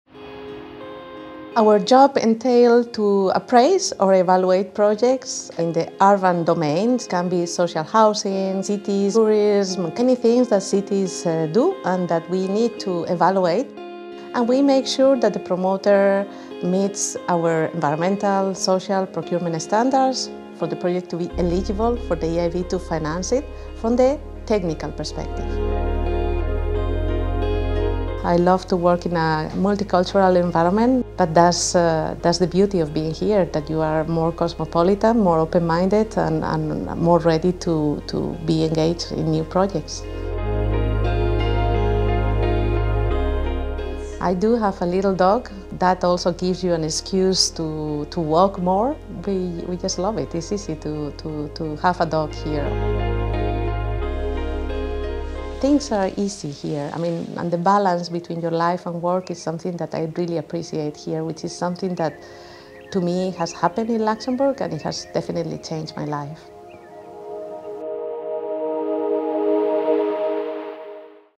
EIB people: Leonor, Lead Engineer
Meet Leonor, her job at the European Investment Bank entails appraising or evaluating projects in the urban domain. She likes the multicultural environment inside the EIB.
Follow her in Luxembourg while she explains why she enjoys her work at the European Investment Bank.